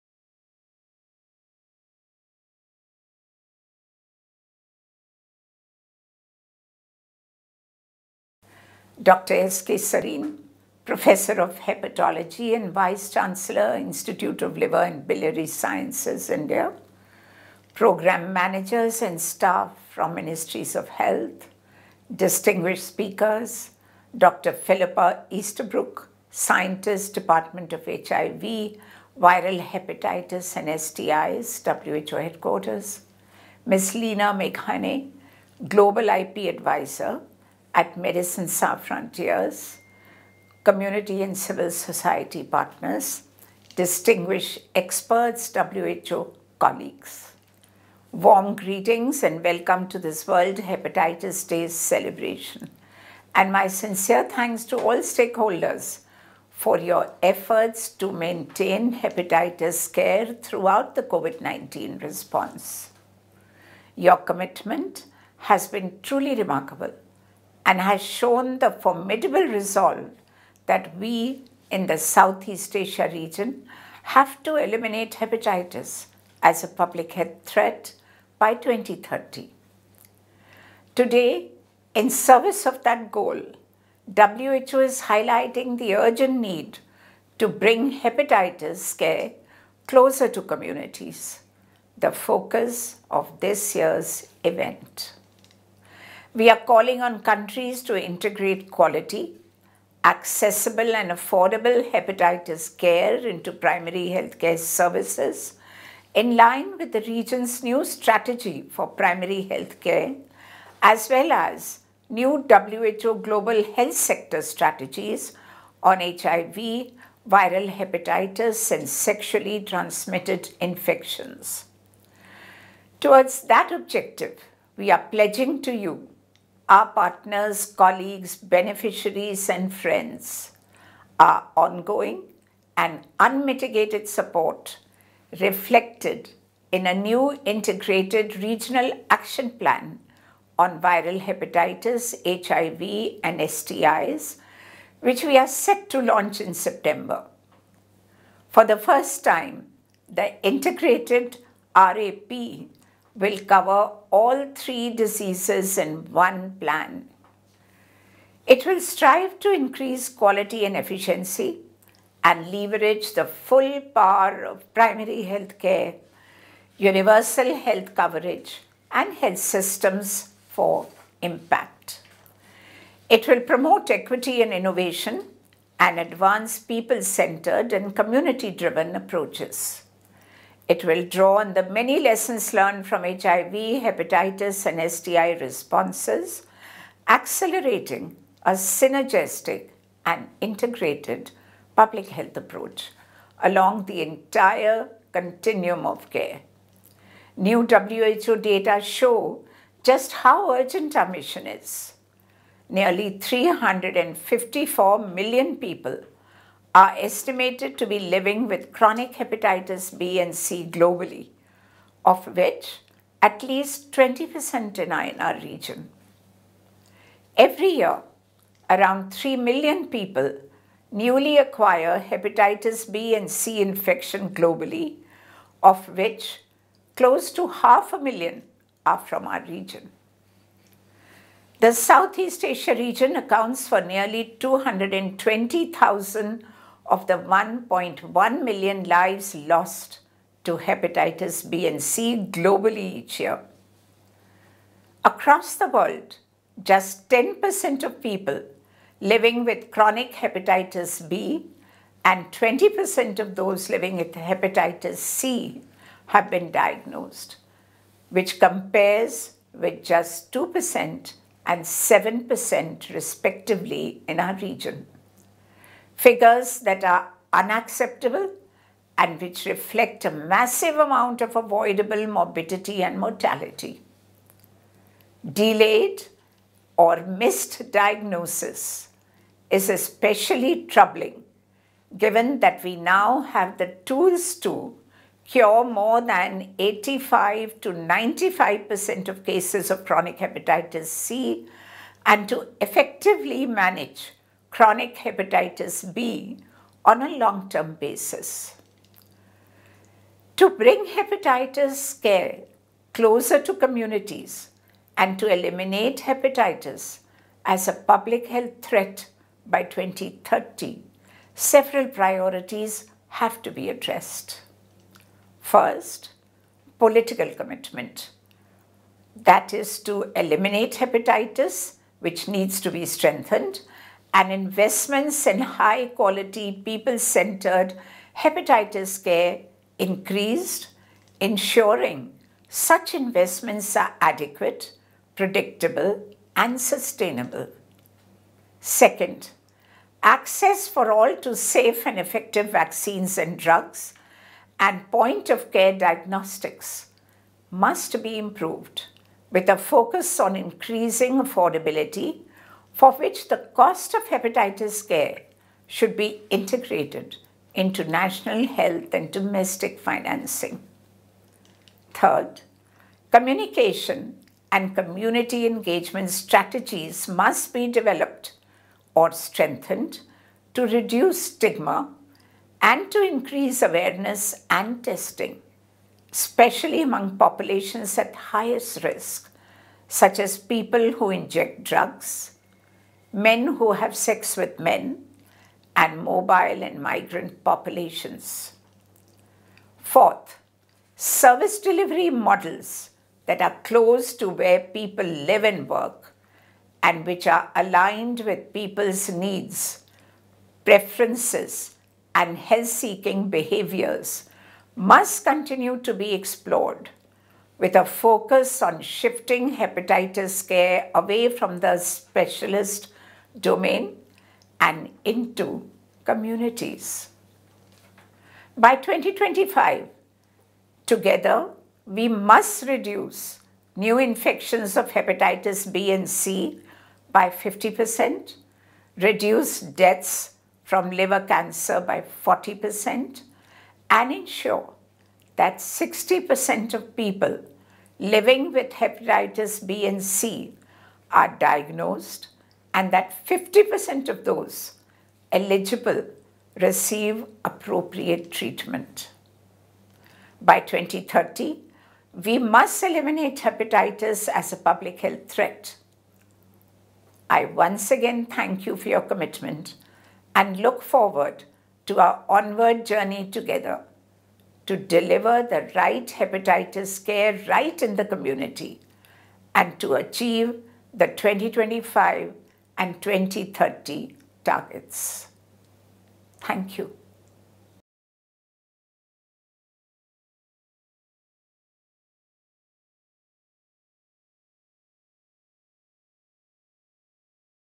Regional Director's remarks on World Hepatitis Day
On WorldHepatitisDay, Dr Poonam Khetrapal Singh, Regional Director, highlights the urgent need to bring hepatitis care closer to communities.